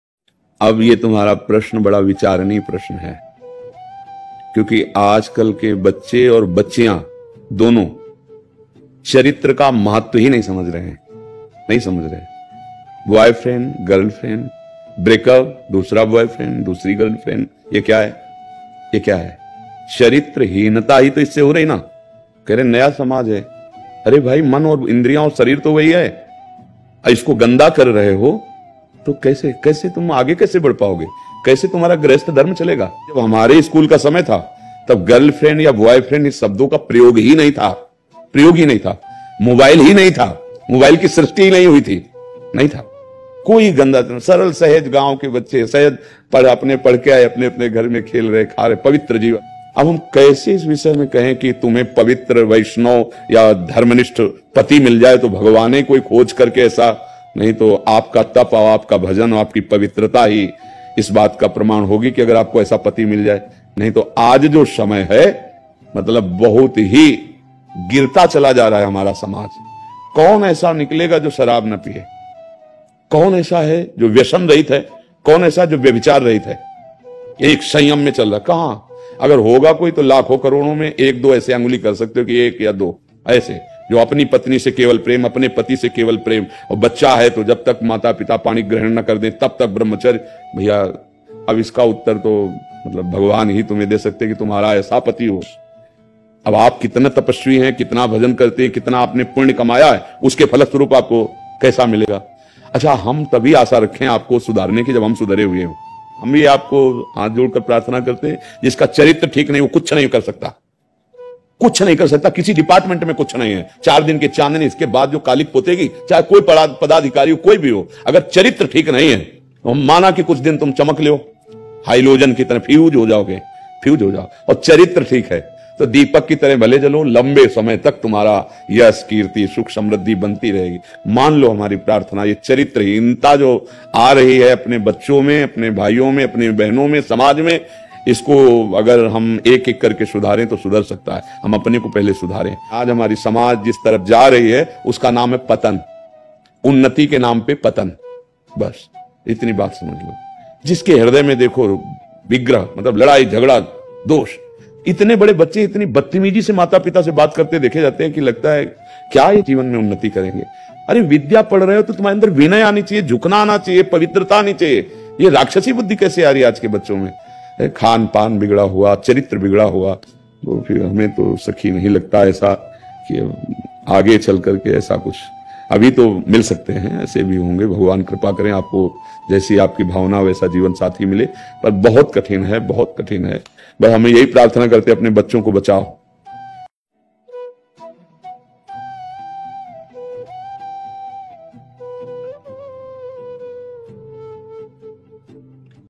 कैसे मिलेगा अच्छा जीवन साथी? how to find true friends
hello friends today video
कैसे मिलेगा अच्छा जीवन साथी? hit premanand govind saran Maharaj ji
premanand ji Maharaj ne bataya achhe jeevan sathi ki pahchan
ye baaten jarur sune akhir esa kyaa bataya Maharaj ji ne